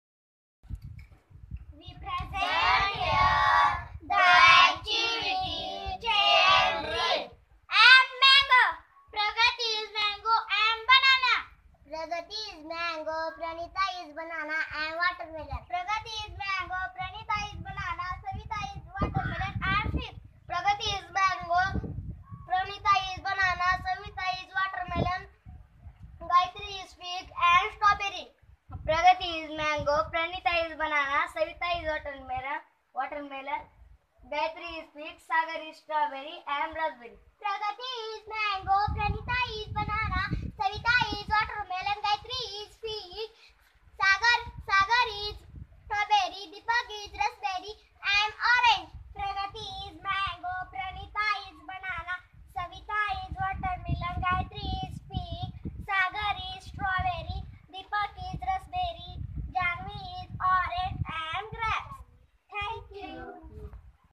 Chain drill activity
Chain drill activity enacted by students in Jogala